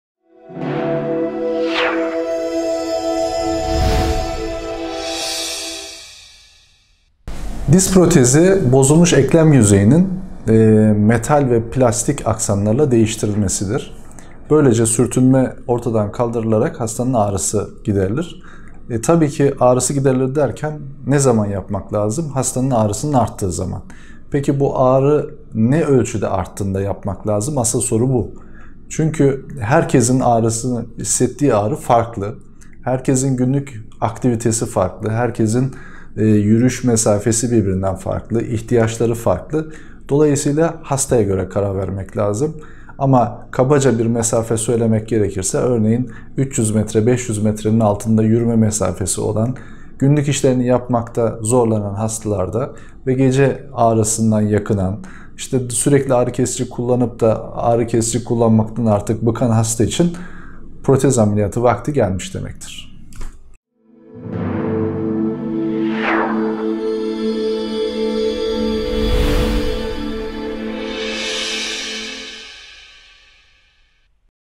Diz protezi ne zaman yapılır? I Prof. Dr. Levent ALTINEL
Merhaba, ben Levent Altınel. Bu videomda size  diz protezi ne zaman yapılır, neye göre diz protezi ameliyatı kararını alacağınızı anlattım.
Video Etiketleri : diz, diz protezi, prof dr levent altınel, artroz, diz protezi ne zaman yapılır, ne zaman diz protezi olmalıyım